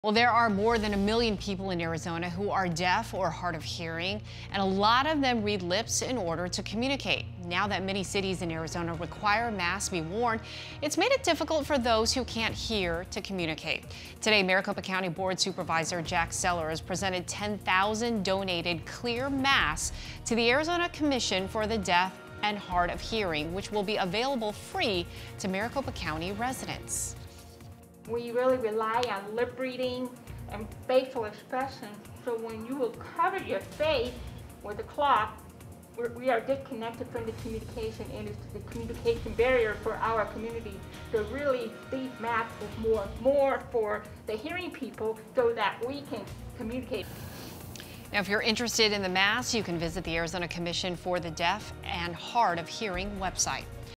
10,000 clear masks donated for deaf Arizonans
10,000 donated, clear masks to the Arizona Commission for the Deaf and Hard of Hearing will help the over 1,000,000 deaf or hard of hearing residents of the state.